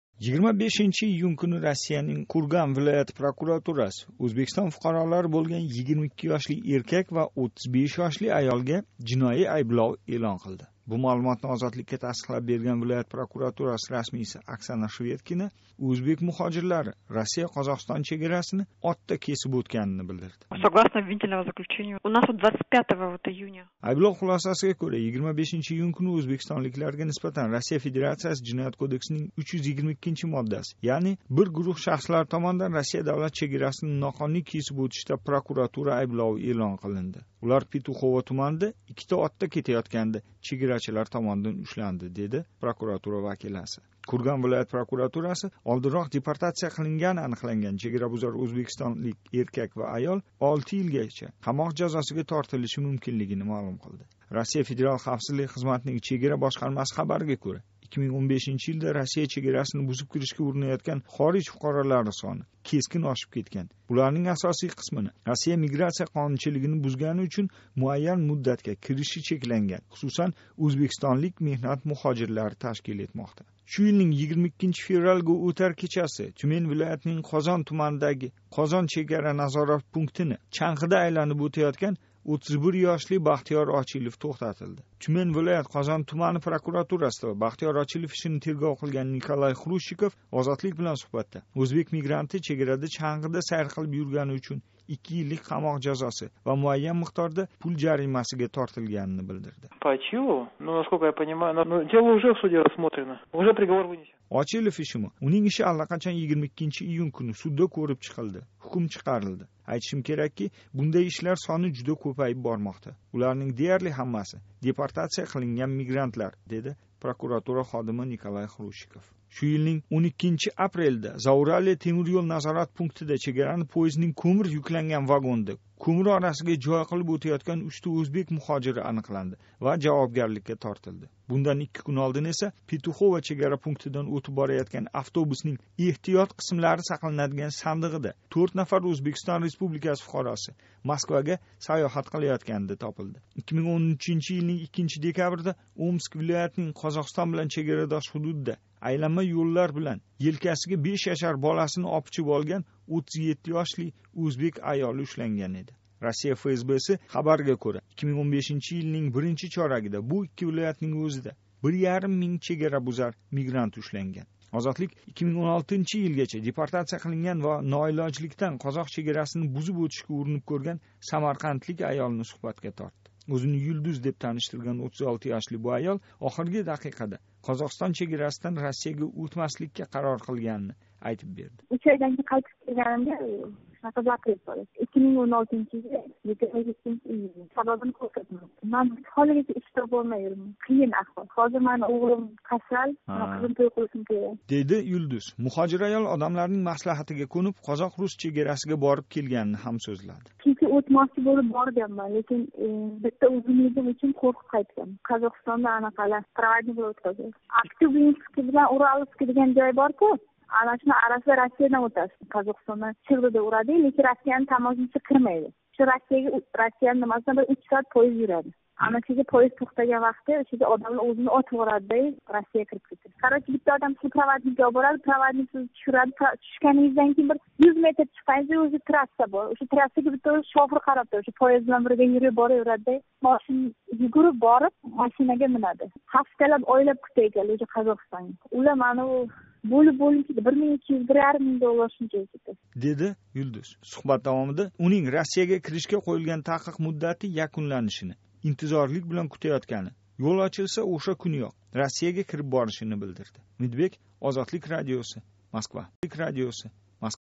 Россия чегарасини бузаётган муҳожирлар кўпаймоқда
Жорий 2015 йилда Россия Федерацияси чегарасини ноқонуний кесиб ўтишга уринаётган меҳнат муҳожирлари сони кескин кўпайиб кетди. Россия чегарачилари Россиянинг айниқса Қозоғистон билан чегарадош жанубий ҳудудлар орқали киришга интилаётган мигрантларнинг аксар қисмини мамлакатдан миграция қонунчилигини бузгани учун депортация қилинганлар ташкил этаётганини таъкидлаяпти.

“Чегарани отда кесиб ўтди!”

25 июн куни Россиянинг Курган вилояти прокуратураси Ўзбекистон фуқаролари бўлган 22 ёшли эркак ва 35 ёшли аёлга жиноий айблов эълон қилди.

Бу маълумотни Озодликка тасдиқлаб берган вилоят прокуратураси расмийси Оксана Шведкина ўзбек муҳожирлари Россия-Қозоғистон чегарасини отда кесиб ўтганини билдирди.

- Айблов хулосасига кўра, 25 июн куни ўзбекистонликларга нисбатан Россия Федерацияси жиноят Кодексининг 322 моддаси 3 қисми, яъни олдиндан келишилган ҳолда бир гуруҳ шахслар томонидан Россия давлат чегарасини ноқонуний кесиб ўтишда прокуратура айблови эълон қилинди. Улар Петухово туманида иккита отда кетаётганида чегарачилар томонидан ушланди, деди прокуратура вакиласи.

Курган вилоят прокуратураси олдинроқ депортация қилингани аниқланган чегарабузар ўзбекистонлик эркак ва аёл олти йилгача қамоқ жазосига тортилиши мумкинлигини маълум қилди.